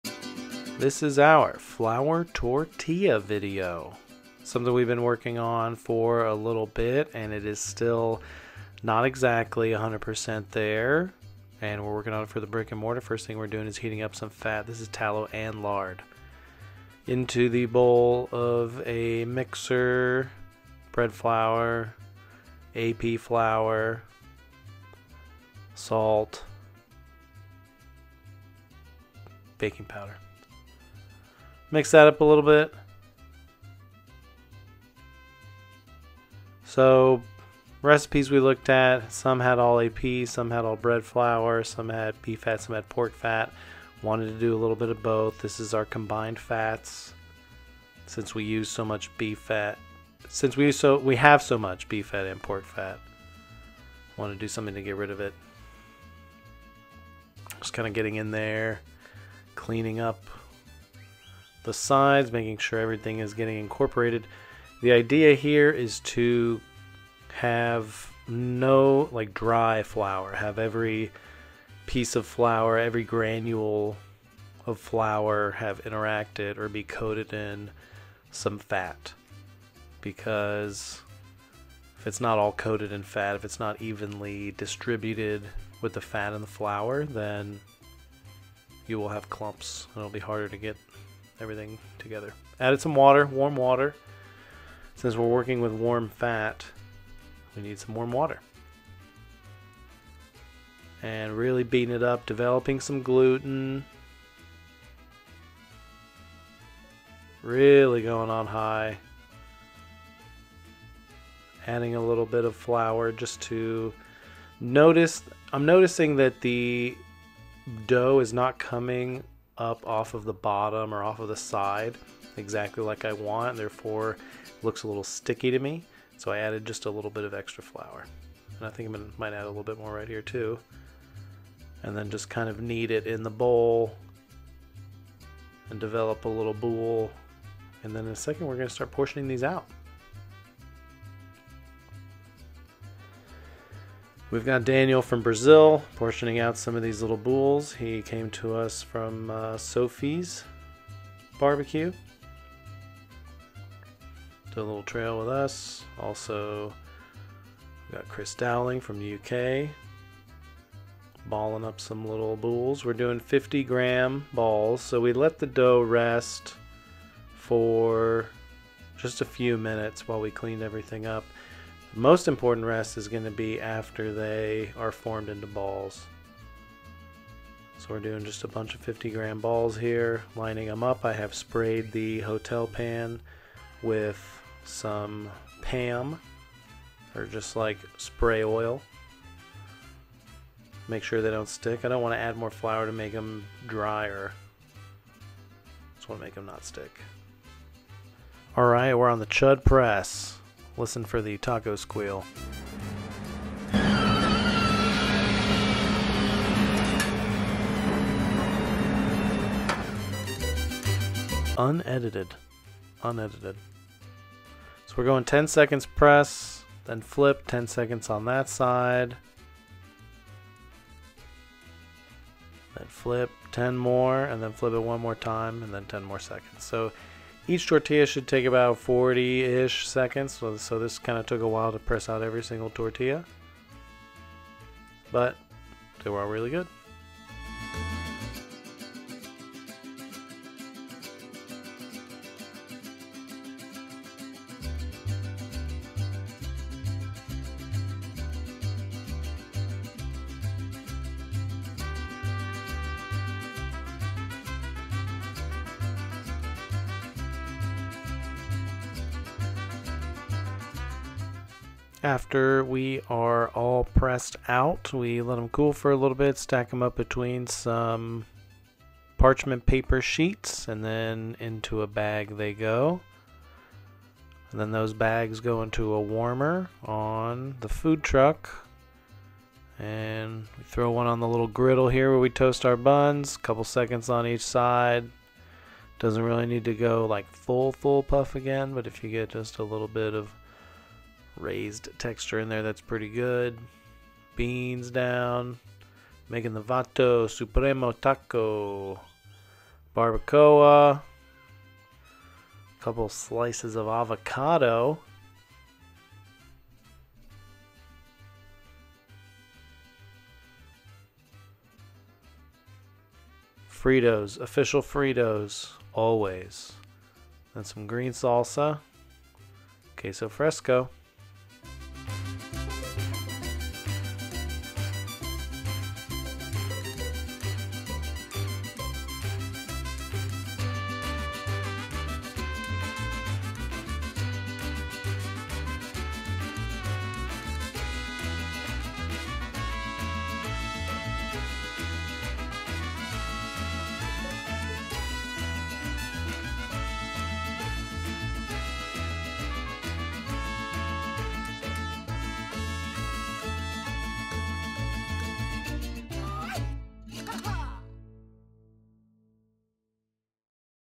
Flour Tortillas with LeRoy and Lewis
Original publish date: 5/21/23.

Evan makes flour tortillas. Kevin makes a Vato Supremo Taco.